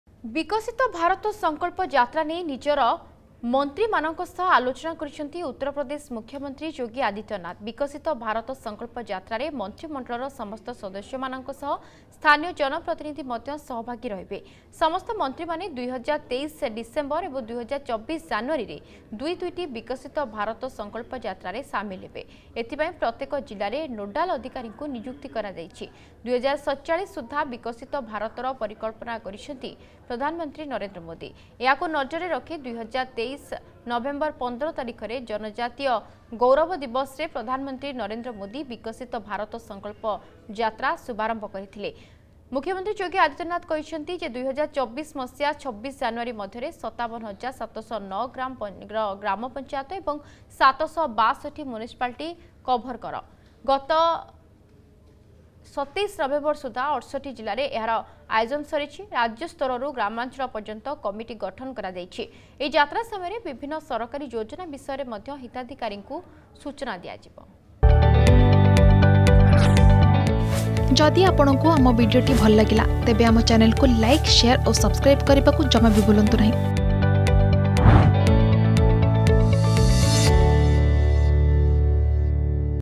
Viksit Bharat Yatra Promoting Prime Minister’s Growth Vision: Yogi Adityanath
ବିକଶିତ ଭାରତ ସଂକଳ୍ପ ଯାତ୍ରା ନେଇ ନିଜର ମନ୍ତ୍ରୀମାନଙ୍କ ସହ ଆଲୋଚନା || UP CM Yogi Adityanath